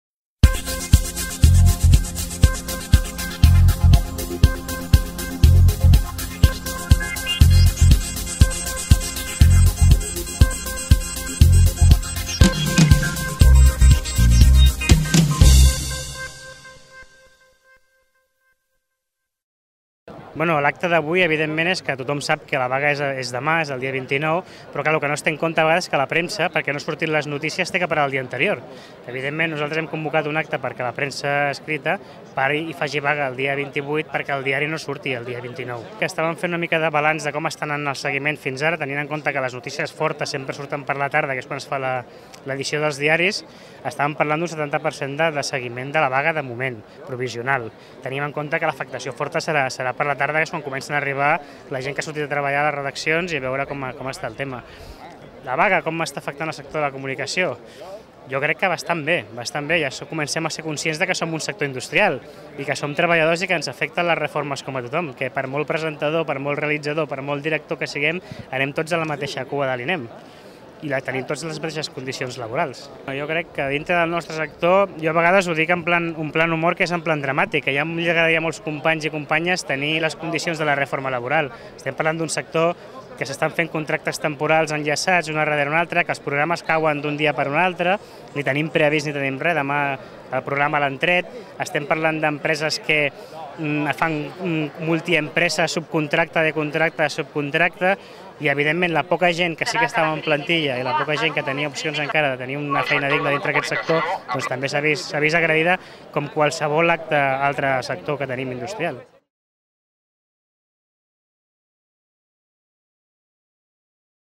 Concentració periodistes vaga general
El sector de premsa escrita està fent vaga avui dia 28. Aquest matí ha tingut lloc a la plaça Sant Jaume de Barcelona, un acte on s'ha llegit el manifest del col·lectiu en contra de la reforma laboral. A continuació podeu veure les valoracions realitzades per Pau Ayuso.